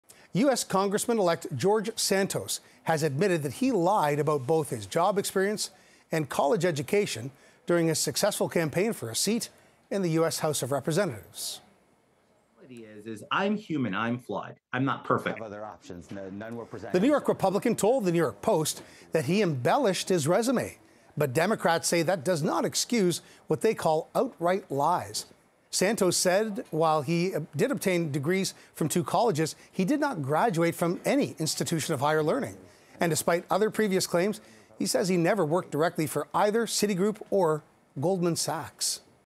U.S. congressman-elect George Santos admits he lied about college and work experience
U.S. Congressman-elect George Santos has admitted he lied about both his job experience and college education during his successful campaign for a seat in the U.S. House.